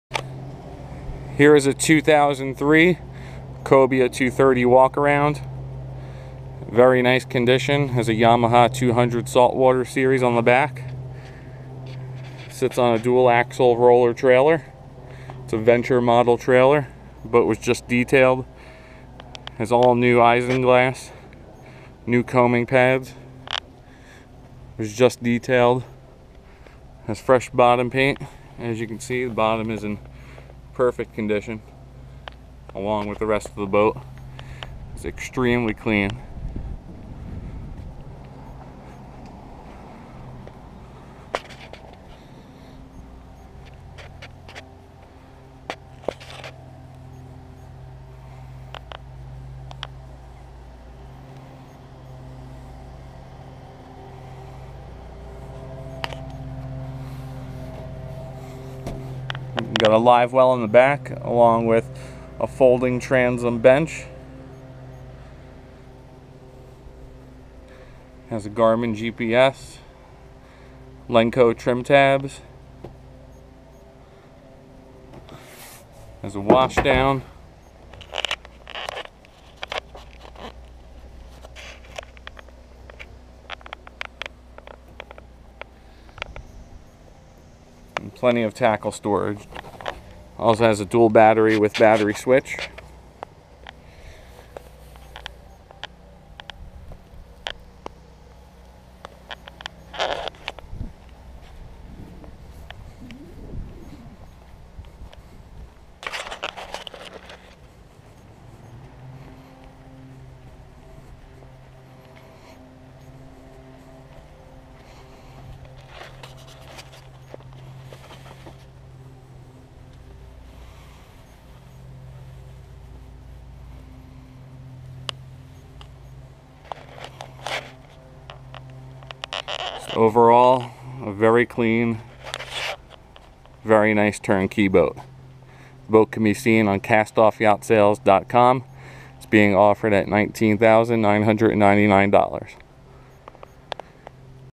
2003 cobia 230 cast off yacht sales
mint condition
200hp OX66
roller trailer
garmin gps
new enclosure